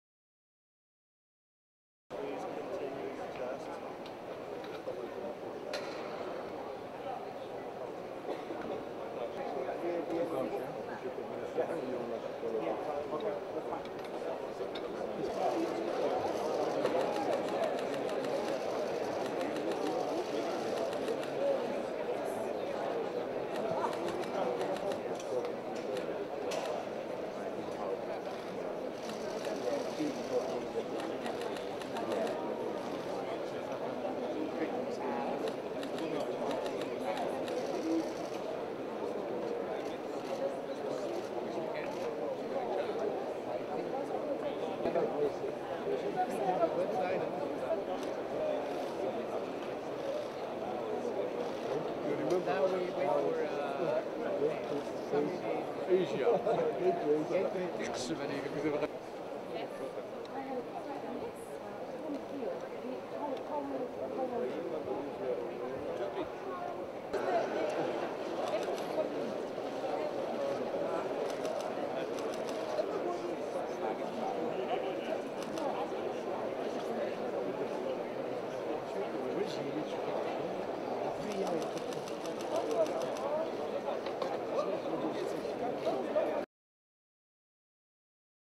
Președintele Klaus Iohannis, la summitul securităţii nucleare
Președintele Klaus Iohannis participă vineri, la Washington, la sesiunea de deschidere a summitului pe tema securității nucleare.